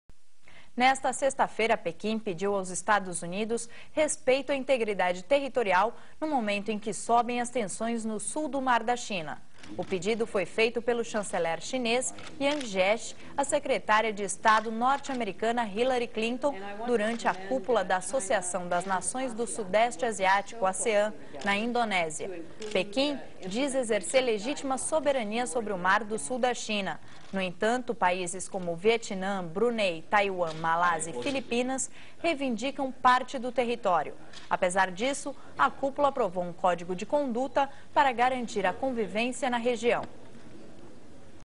Pequim exige respeito à soberania territorial a Washington
Nesta sexta-feira, Pequim pediu aos EUA respeito à integridade territorial no sul do mar da China. O pedido foi feito pelo chanceler chinês à Secretária de Estado Hillary Clinton. teleSUR